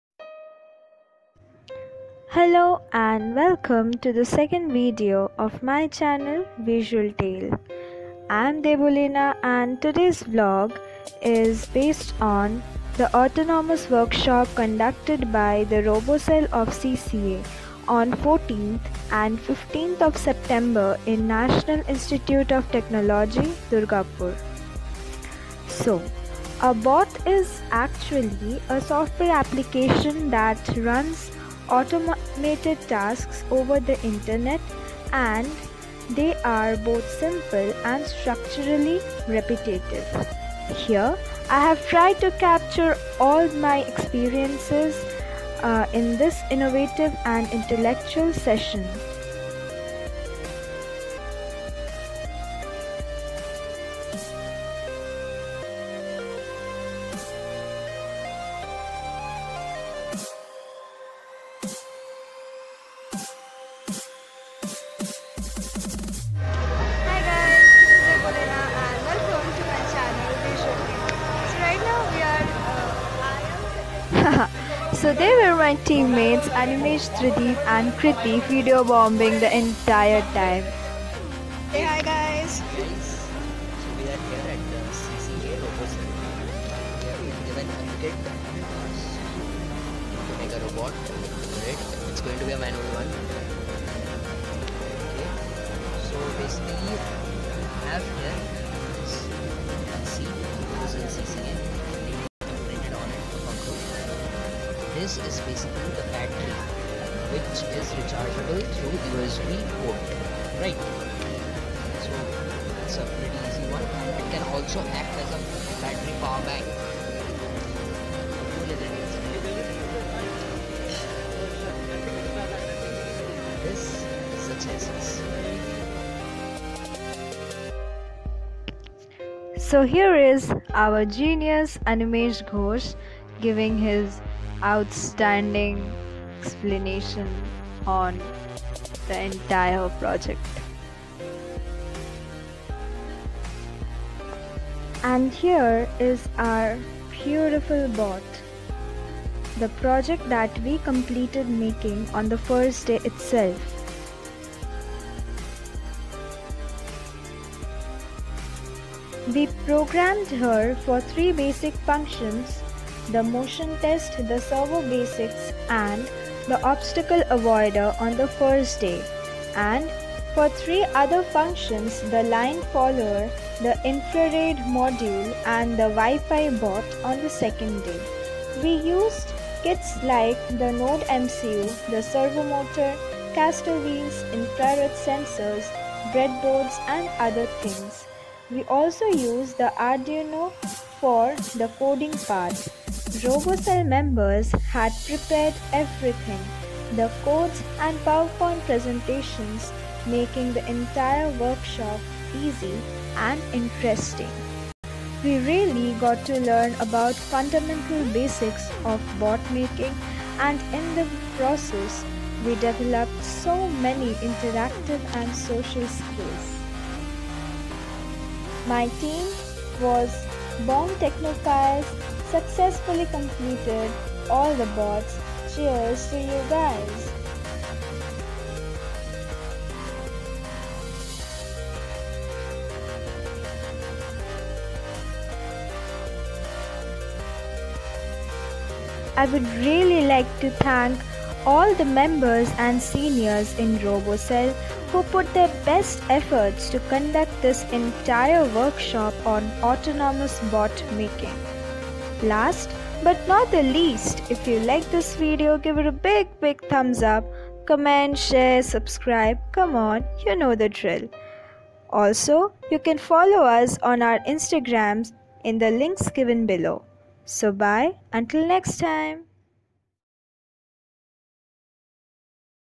ROBOCELL VLOG #2
Hey guys!!

Welcome to my second vlog based on The Autonomous Workshop conducted by the RoboCell of CCA (Centre for Cognitive Activities) in National Institute of Technology, Durgapur. It was conducted in our college on 14th and 15th September at the Student Activity Center (SAC) and made us acquainted with the world of robots. The workshop focussed on developing bots and their working code. Robocell members also provided us with a start away kit with which we developed functions like the Line Follower, the Servo Motor and many other interesting things.